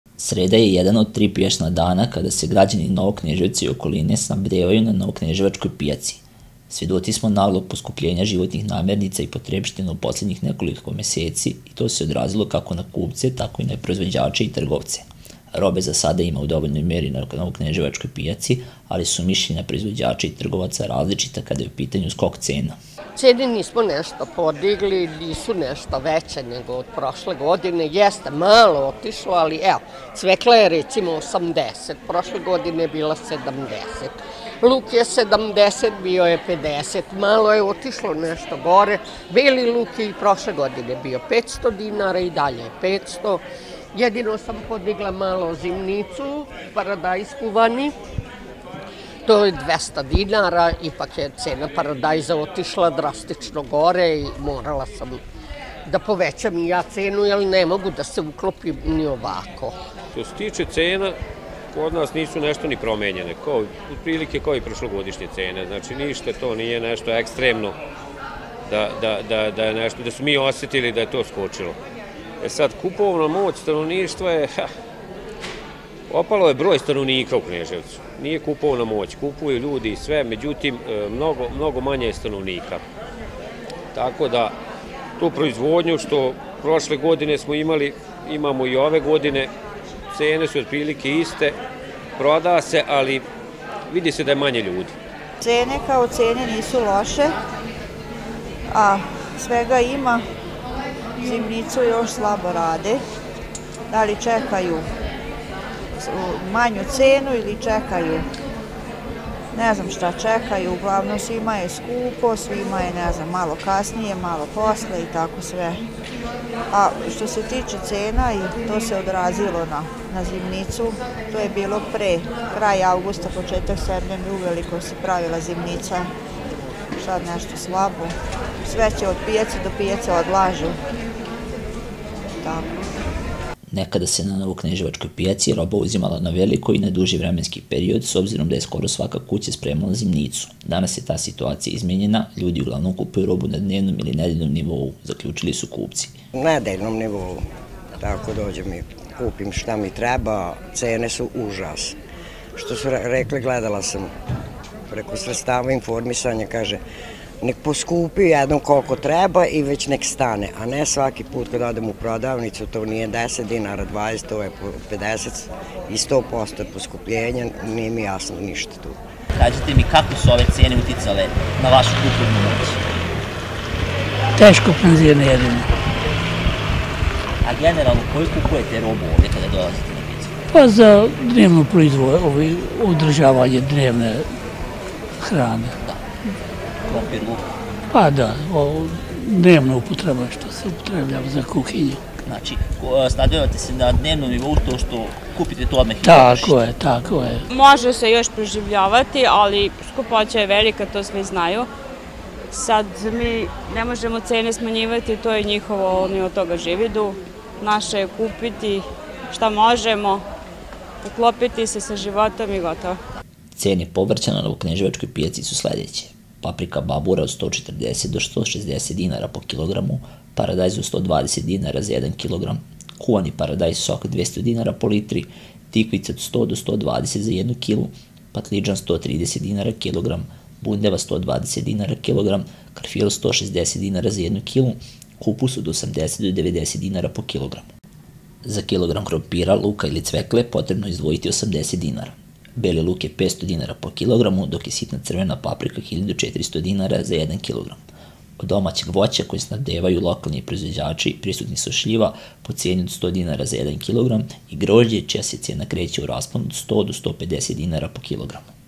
Novokneževački pijačni barometar
Sreda je jedan od tri pijačna dan kada se građani Novog Kneževca i okoline snabdevaju na novokneževačkoj pijaci. Svedoci smo naglog poskupljenja životnih namirnica i potrepština u poslednjih nekoliko meseci, i to se odrazilo kako na kupce, tako i na proizvođače i trgovce. Robe za sada ima u dovoljnoj meri na novokneževačkoj pijaci, ali su mišljenja proizvođača i trgovaca različita kada je u pitanju skok cena.
  Nekada se na novokneževačkoj pijaci roba uzimala na veliko i na duži vremenski period, s obzirom da je skoro svaka kuća spremala zimnicu. Danas je ta situacija izmenjena, ljudi uglavnom kupuju robu na dnevnom ili nedeljnom nivou, zaključili su kupci.
  Cene povrća na novokneževačkoj pijaci su sledeće: parpika babura od 140 do 160 dinara po kilogramu, paradajz od 120 dinara za jedan kilogram, kuvani paradajz sok 200 dinara po litri, tikvice od 100 do 120 za jednu kilu, patlidžan 130 kilogram, bundeva 120 dinara kilogram, karfiol 160 dinara za jednu kilu,  kupus od 80 do 90 dinara po kilogramu. Za kilogram krompira, luka ili cvekle potrebno je izdvojiti 80 dinara za jednu kilu. Beli  luk je 500 dinara po kilogramu, dok je sitna crvena paprika 1400 dinara za jedan kilogram. Od domaćeg voća koje snabdevaju lokalni proizvođači  prisutni su šljiva po ceni od 100 dinara za jedan kilogram, i grožđe čija se cena kreće u rasponu od 100 do 150 dinara po kilogramu.